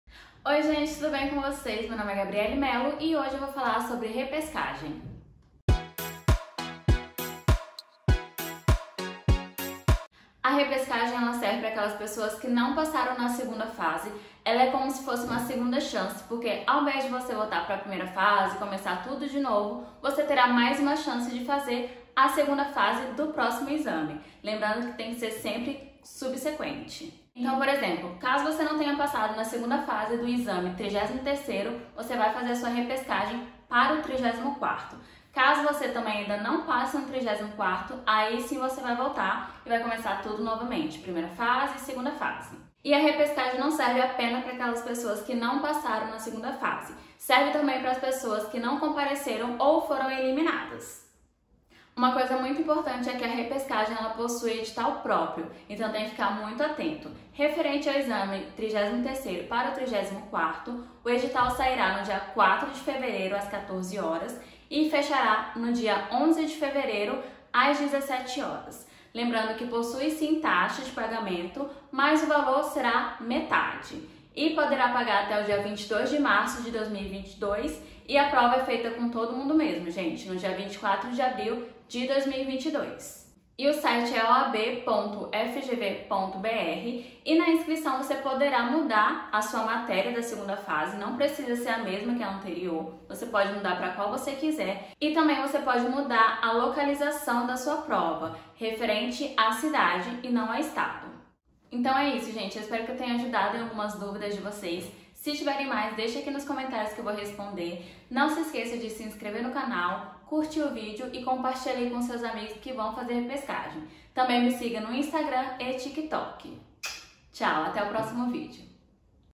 COMO FUNCIONA A REPESCAGEM OAB? | Pode mudar a matéria?
Oi gente, o vídeo de hoje é para aquelas pessoas que precisam enfrentar mais uma vez a 2º fase do exame de ordem. Não se preocupe que nesse vídeo falei as coisas mais importantes que precisa saber.